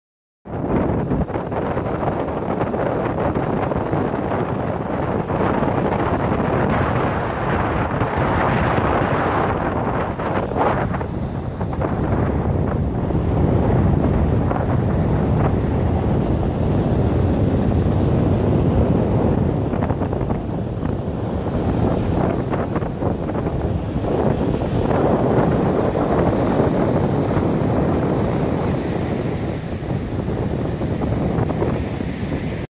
Looking across Lake Michigan towards Chicago
Indiana Dunes National Lakeshore/State Park.

6 May 2017 - Porter, Indiana